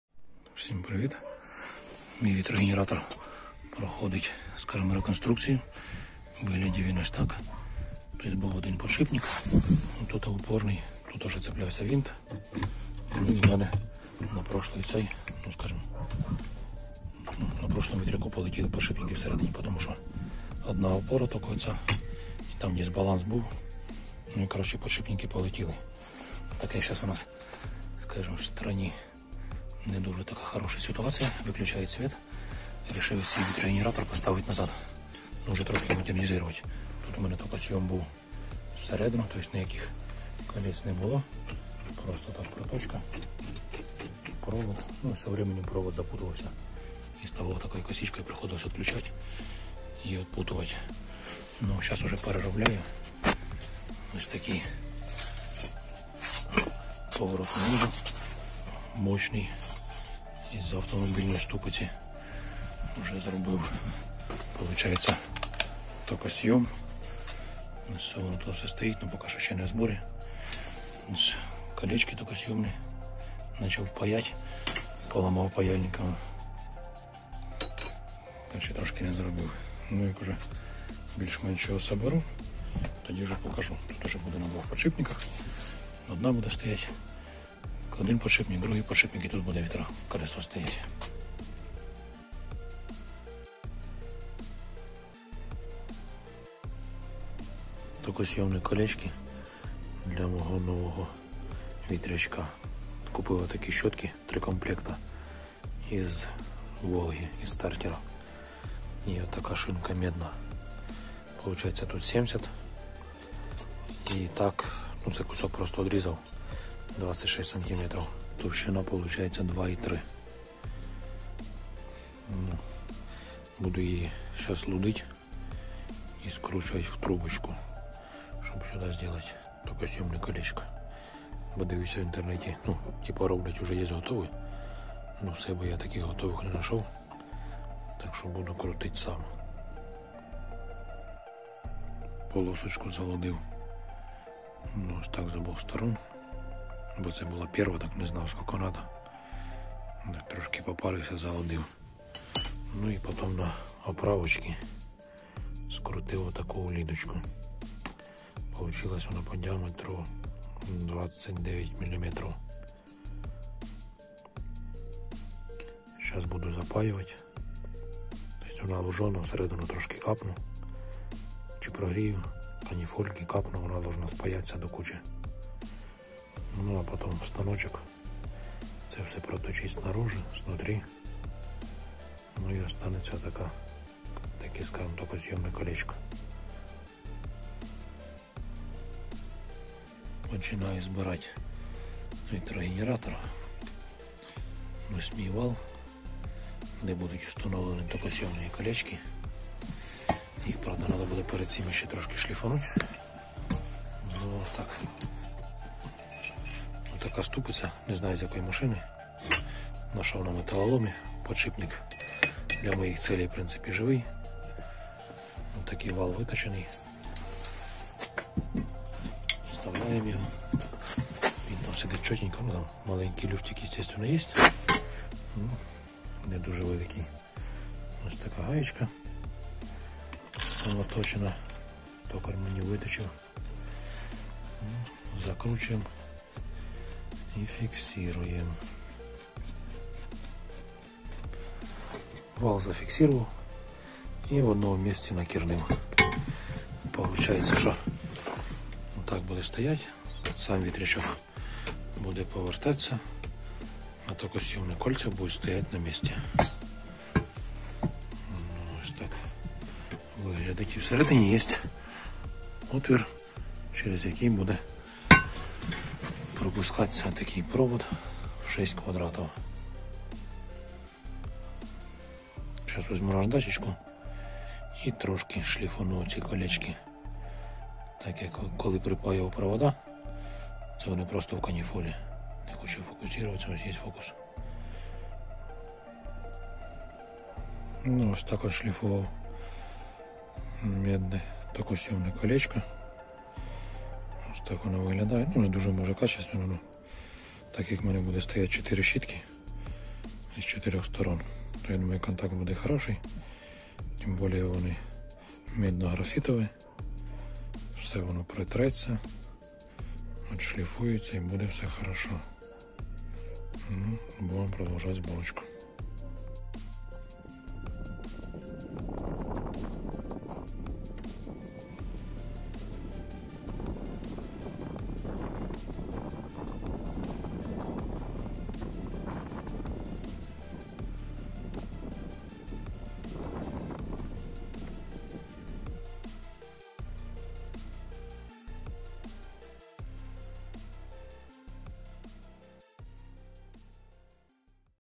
Вітрогенератор своїми руками, глибока модернізація, щіточний вузол, виготовлення мідних кілець
Дійшла черга до модернізації мого вітрогенератора. Було перероблено майже все, окрім лопастей і самого моторколеса, яке я використовую в якості генератора.
Це перша вступна частина, де є короткий огляд конструкції вітрогенератора та я показую як і з яких деталей складається мій токоз'ємний вузол. А саме, я використав щіточний вузол від редукторного стартера Волга, саморобні токоз'ємні кільця, виготовлені з полоси міді, та вал, на якому це все встановлюється.